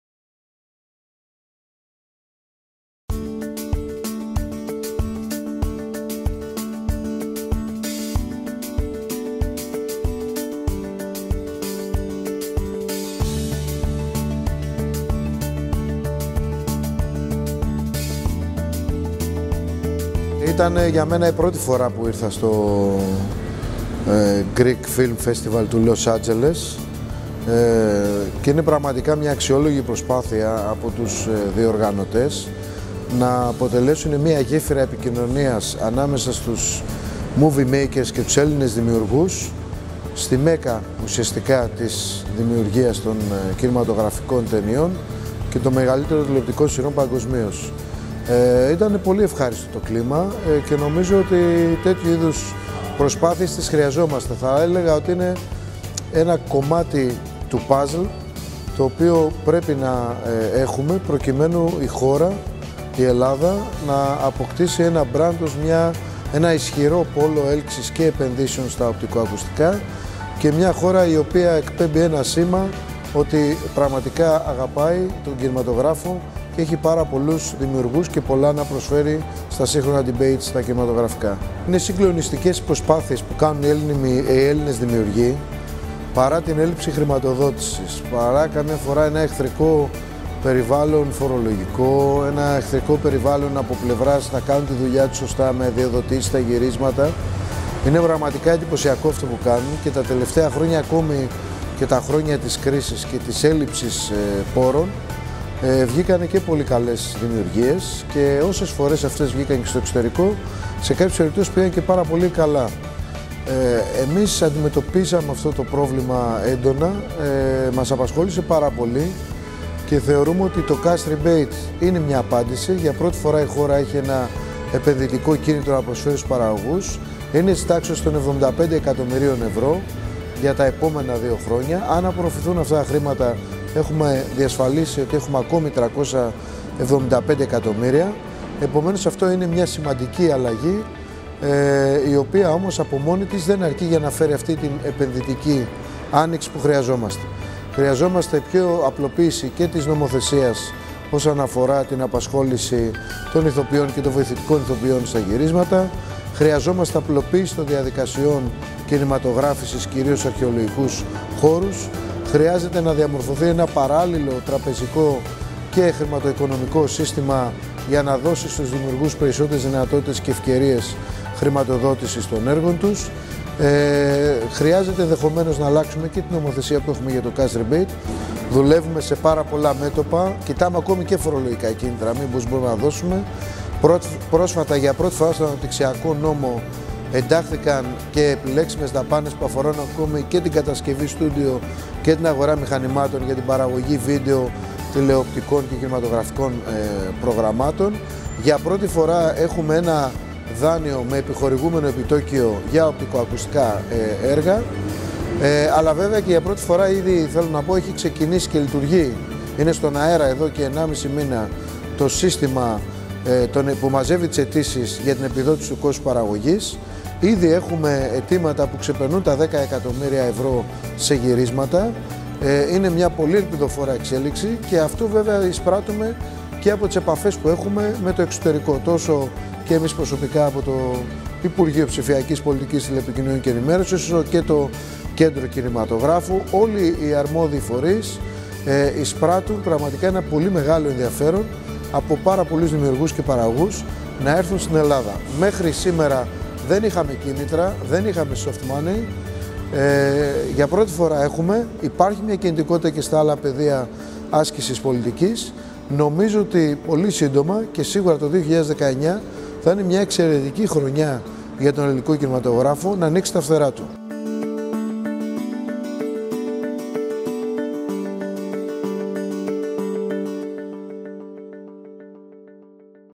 Ο ΓΓΕΕ Λ. Κρέτσος στο 12o Φεστιβάλ Ελληνικού Κινηματογράφου του Λος Αντζελες
06.2018
Ο Γενικός Γραμματέας Ενημέρωσης και Επικοινωνίας Λευτέρης Κρέτσος  βρέθηκε  στο Egyptian Theater  και στο 12 Φεστιβάλ Ελληνικού Κινηματογράφου του Λος Αντζελες. Μιλησε στον  Πάνο Σατζόγλου  για την επιτυχία των διοργανωτών του φεστιβαλ που προωθούν τους Έλληνες κινηματογραφιστές  στη μέκκα του κινηματογράφου, και αναφέρθηκε στην προώθηση  του νέου θεσμικού πλαίσιου  για την ενίσχυση των κινηματογραφικών και τηλεοπτικών παραγωγών στην Ελλάδα.